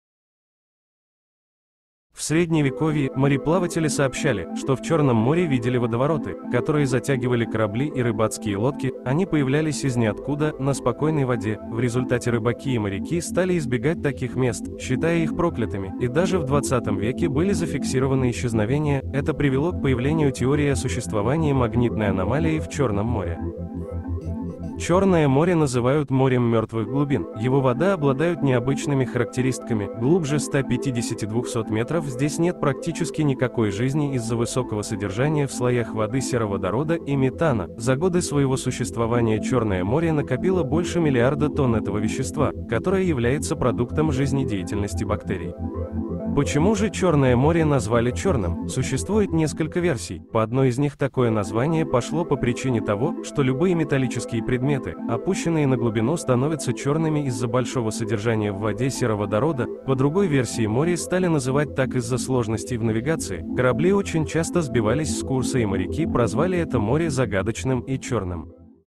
ТОП 3 ТАЙНЫ ЧЁРНОГО МОРЯ...
Чёрное море привлекает каждый год миллионы туристов и отдыхающих. Ласковое солнце, тёплая морская вода и прекрасные пляжи. Кажется мы знаем всё о Чёрном море. Но и у него есть свои неизвестные и интересные тайны...